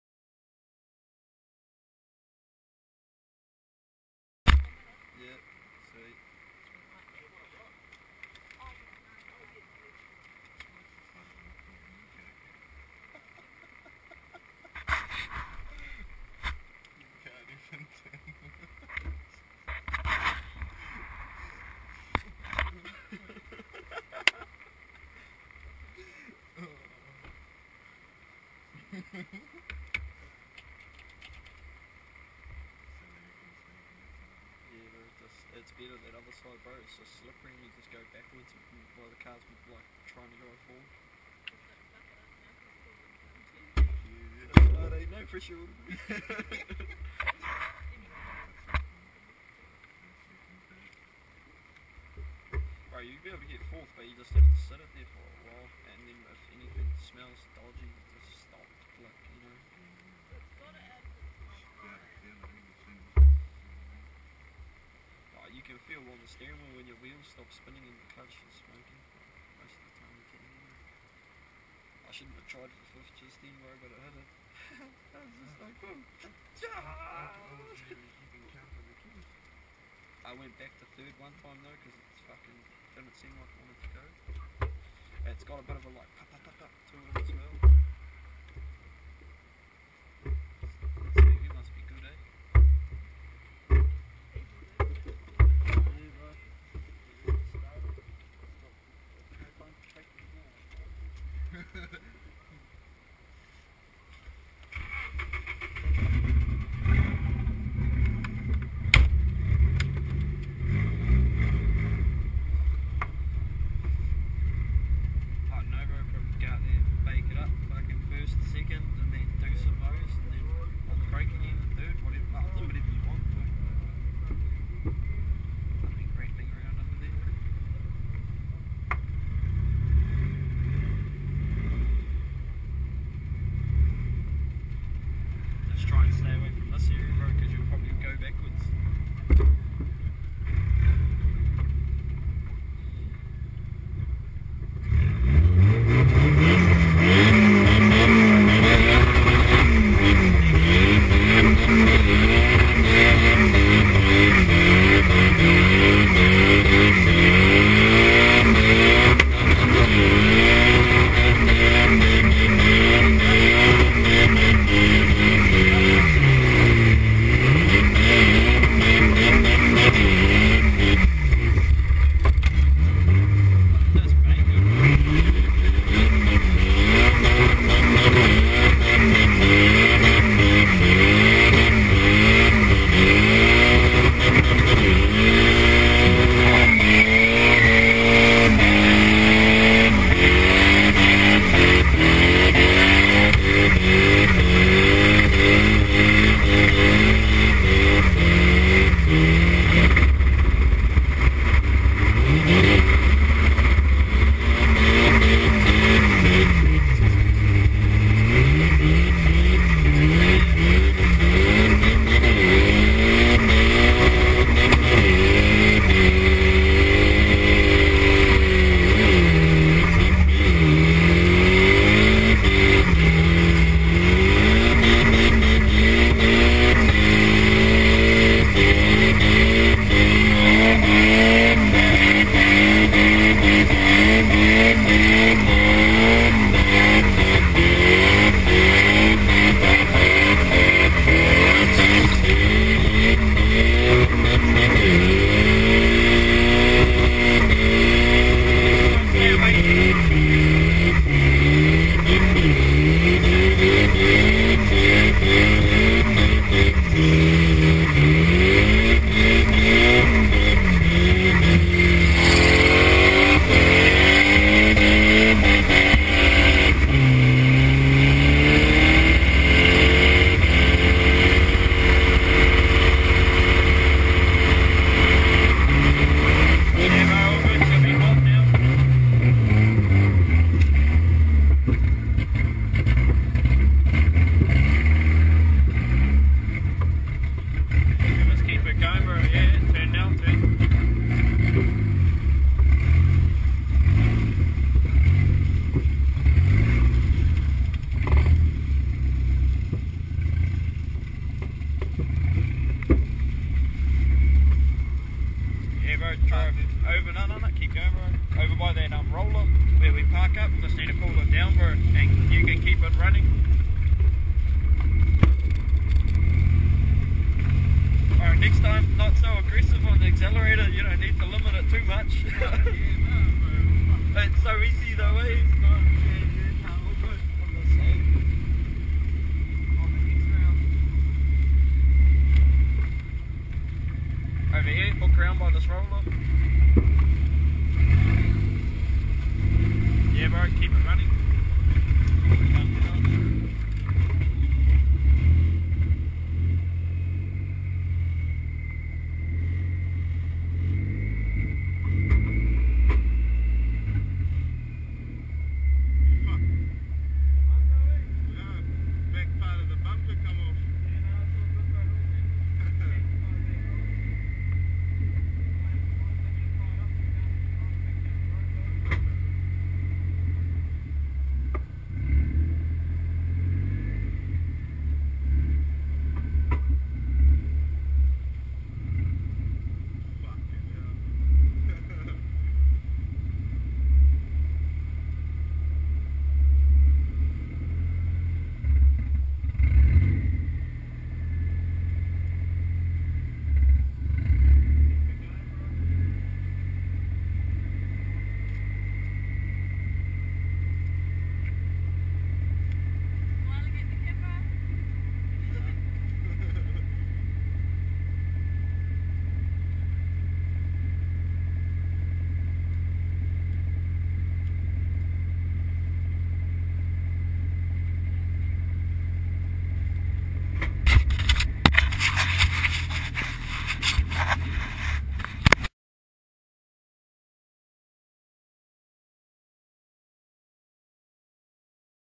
Wiremu And Logan Inside Car Maori TV action Camera 10 June 2011
kaikohe Speedway
Driver: Wiremu.
Passenger: Logan
Filmed by Maori TV - action cam
see logan trying to get out on the cameras view